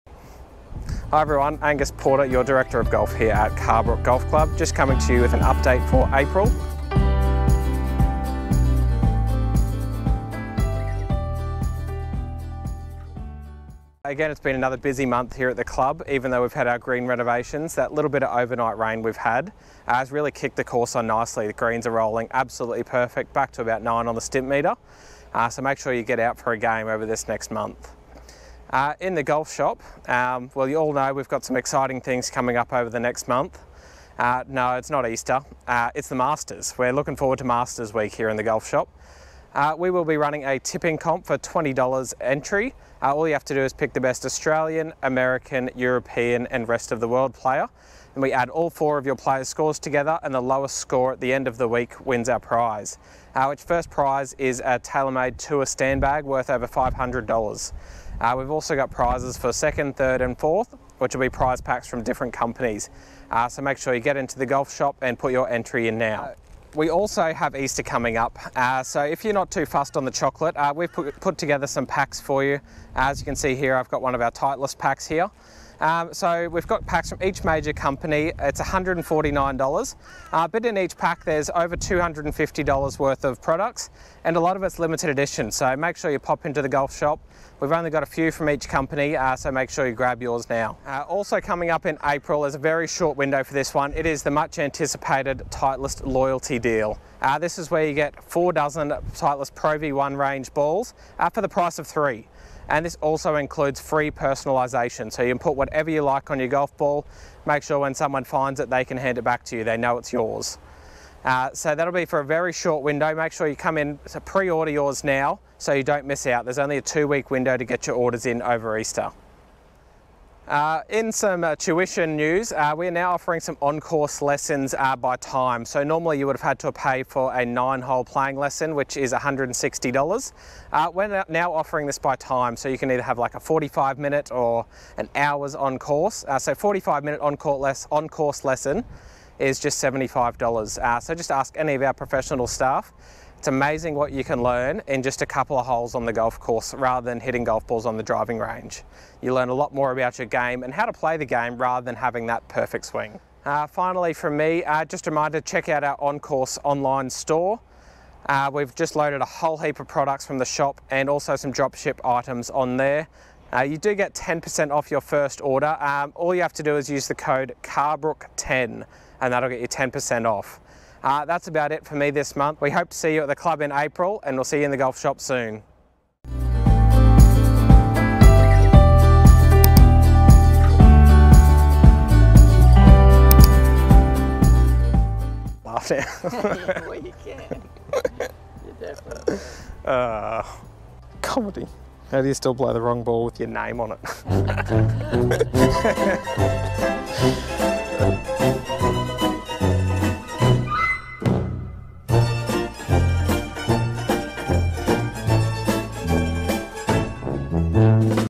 The Golf Shop | April Update
Angus talks us through what is happening in and around the Golf Shop this month. Touches on The Masters, Easter, changes to lesson availability and how to save money in the shop this month!